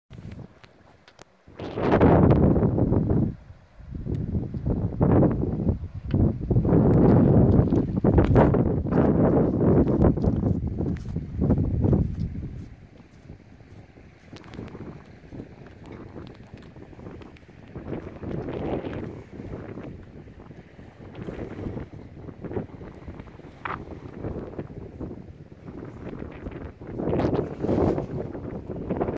kasivaren wilderness area, kilpisjarvi, finland
hiking in Kasivaren wildernes area near Kilpisjarvi, Finland